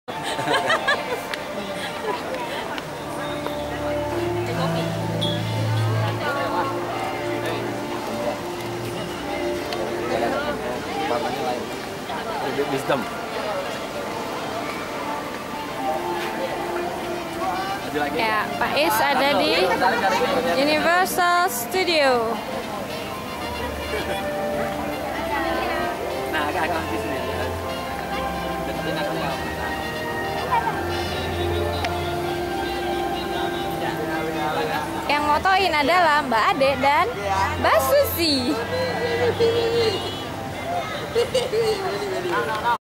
Jalan jalan ke Universal Studio Singapore
jalan jalan sore sama temen temen ke Universal Studio Singapore.